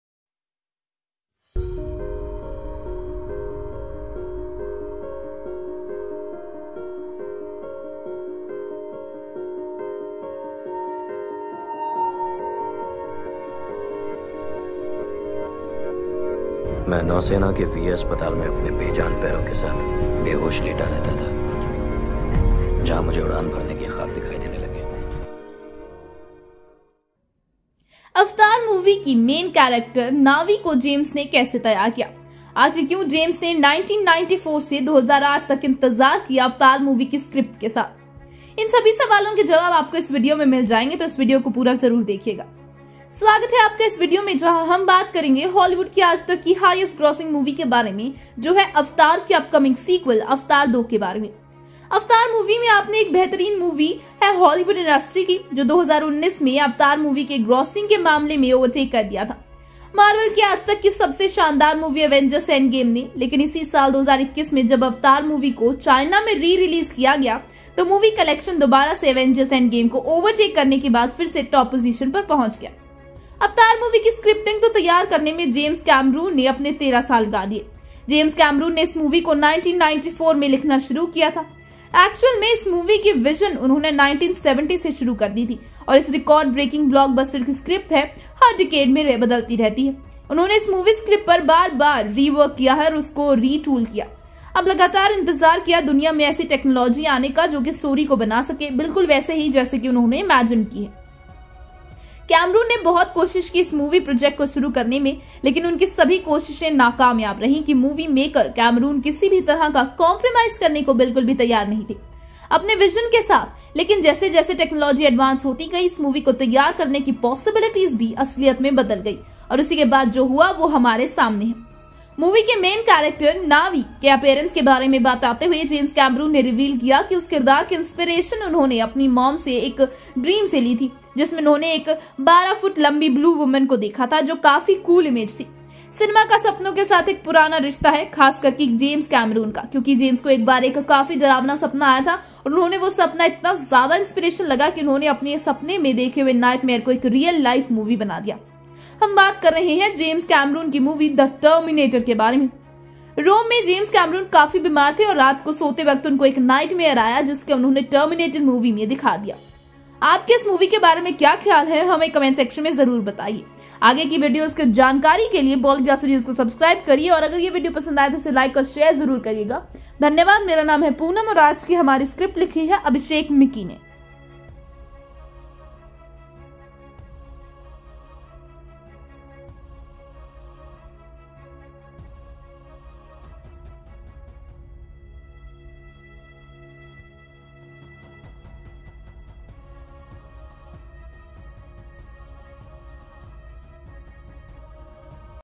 Avatar 2- 31 UNBELIEVABLE facts |James Cameron | Sci-Fi | Action Movie Disney+
13 years after the events of the original film, Jake Sully and Neytiri have formed a family and are doing anything to stay together. They are, however, forced to leave their home and explore the different regions of Pandora when an old threat returns to finish what it started.

“Avatar 2” just got a bit more real with the release of official concept art that is teasing the new worlds and new creatures that await moviegoers in the long-delayed sequel to James Cameron’s 2009 box office phenomenon. The official “Avatar” social media account dropped four exclusive looks at the expanded scope of “Avatar 2,” assuring whatever “Avatar” fans are left around the world that “you won’t just return to Pandora — you’ll explore new parts of the world.” Cameron has been vocal about “Avatar 2” exploring underwater worlds.

James Cameron is currently developing four Avatar sequels. The original movie opened over ten years ago and became the highest grossing movie of all time worldwide (unadjusted for inflation), a title it held for a decade until it was surpassed last summer by “Avengers: Endgame.” Cameron originally wanted to release Avatar 2 in 2014, but the added sequels and the new technology needed for his extensive underwater sequences pushed the film back to December 2020 and then again to December 2022. Cameron said in December that fans should not expect any more delays in terms of the film’s release.

Avatar 2 will reunite Cameron with a large portion of his original cast, including Sam Worthington, Zoe Saldana, and Sigourney Weaver. New cast additions include Kate Winslet, Oona Chaplin, and Vin Diesel.

PLOT: A paraplegic marine dispatched to the moon Pandora on a unique mission becomes torn between following his orders and protecting the world he feels is his home.
FAN MADE

CAST: Sam Worthington, Zoe Saldana, Sigourney Weaver

Editor : Gautam
Script : Abhishek
Anchor : Poonam